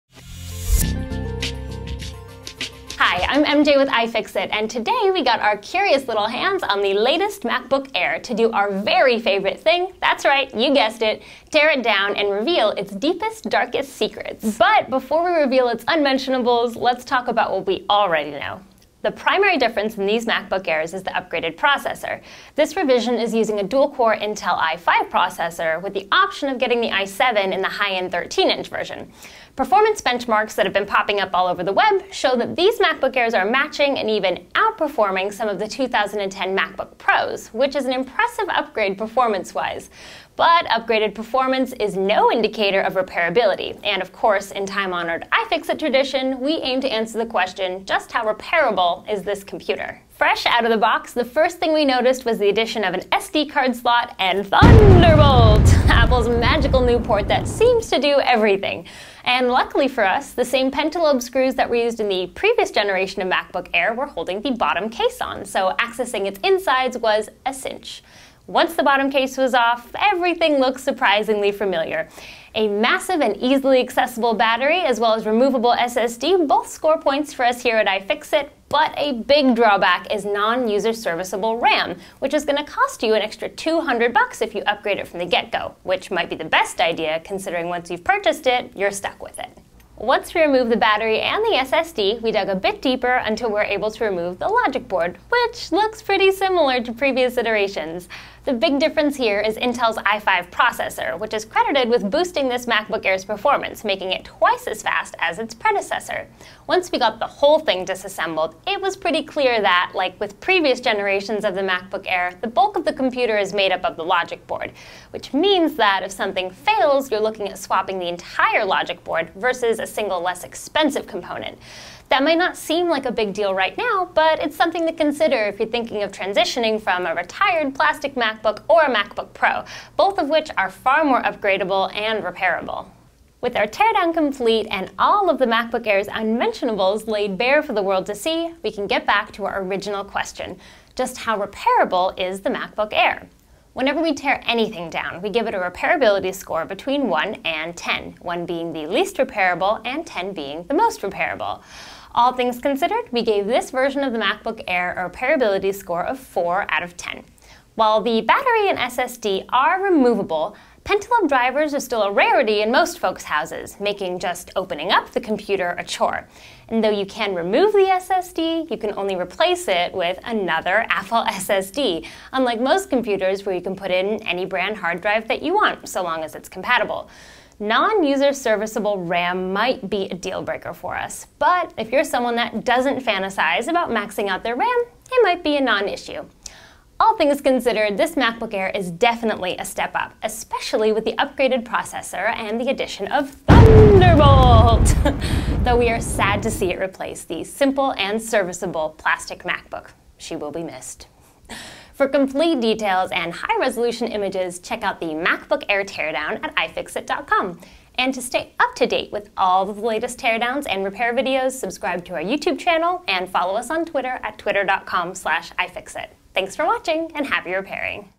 MacBook Air 2011 Teardown Round Up
The Apple-fan rumor mill has been running non-stop in anticipation of a revision to the game-changing MacBook Air. Well, finally, after months of speculation, they have arrived. We got our hands on one and promptly tore it down, to reveal its deepest darkest secrets. This video is a roundup of our findings, and includes our post-teardown repairability analysis.

THUNDERBOLLLLT!